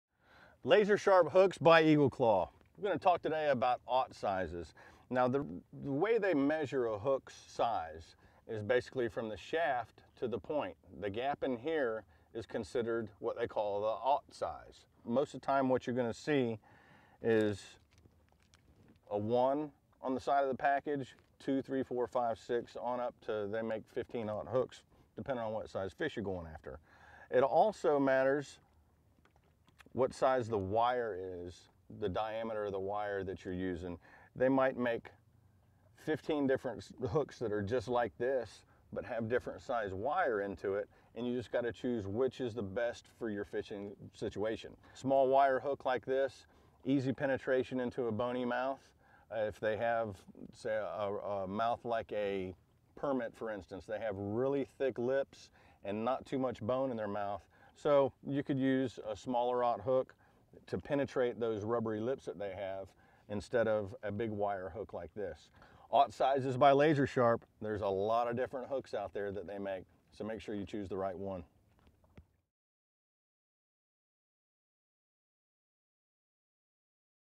Different size Lazer Sharp hooks
Capt. Blair tells us when to use different sizes of Lazer Sharp hooks depending on the situation.